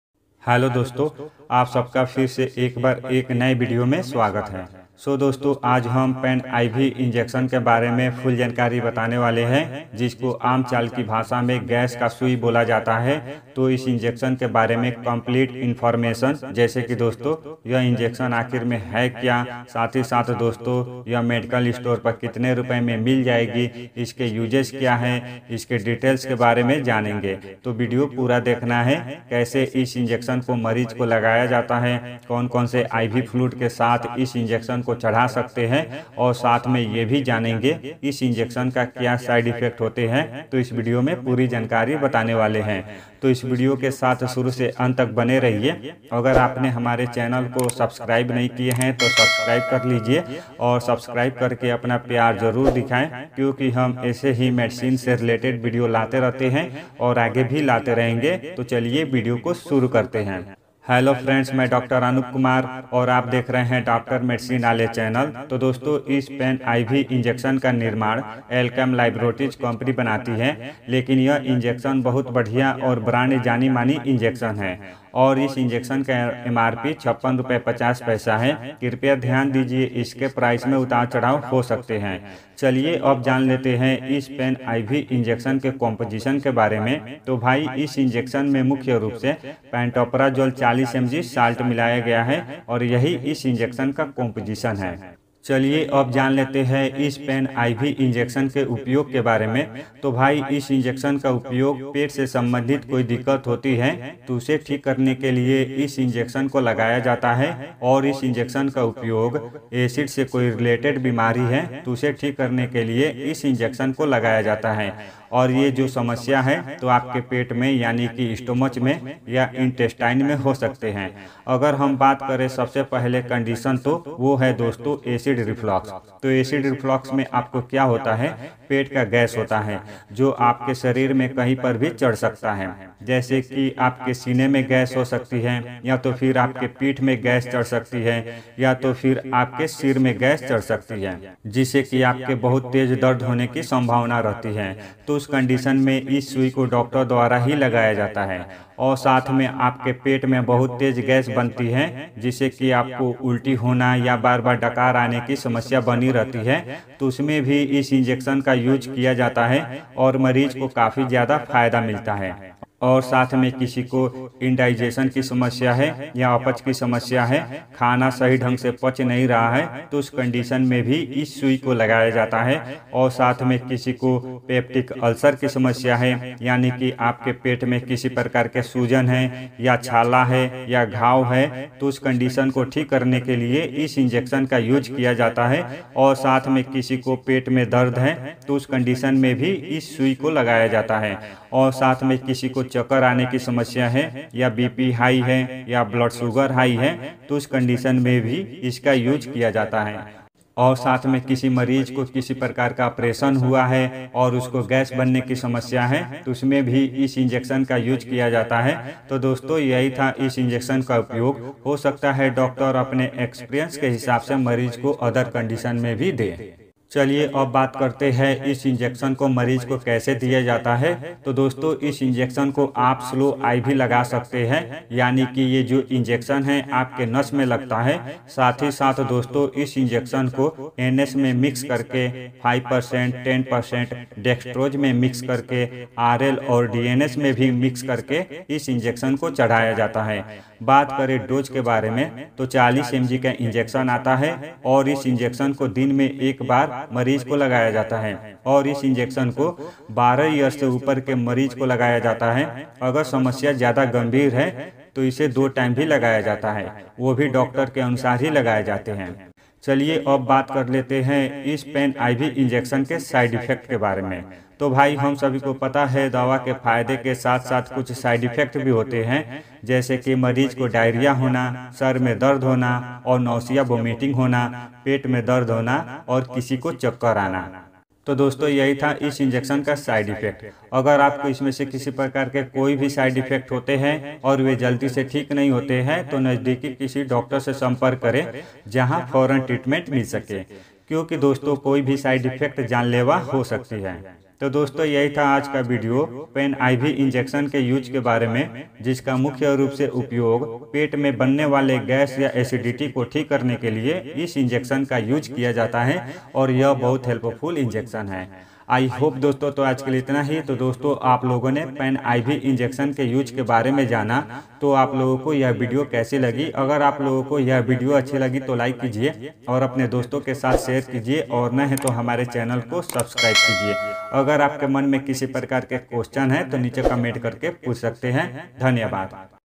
Pan iv injection uses in hindi/pan iv injection ke fayde/pantoprazole injection/uses doses nuksaan
Hello dosto is video me pan iv injection ke fayde aur nuksaan ke bare me full information bataya hai aur iske sath me ye bhi bataya hai ki isko kaise lgaya jata hai aur kab lagaya jata hai to dosto is video ko pura last tak dekhe aur channel ko subscribe karna na bhule

By - Dr Anup Kumar